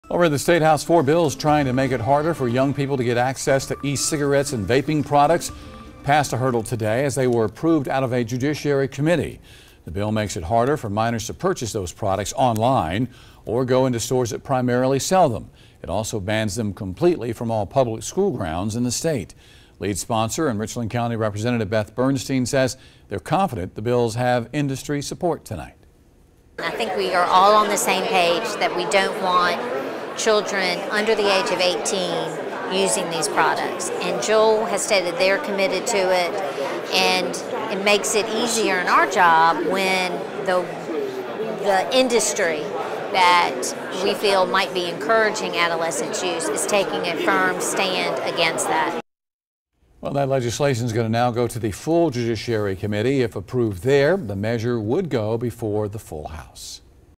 Bills advance to make e-cigarettes harder for minors to get
Four bills trying to make it harder for young people to get access to e-cigarettes and vaping products, passed a major hurdle today as they were approved out of a judiciary subcommittee.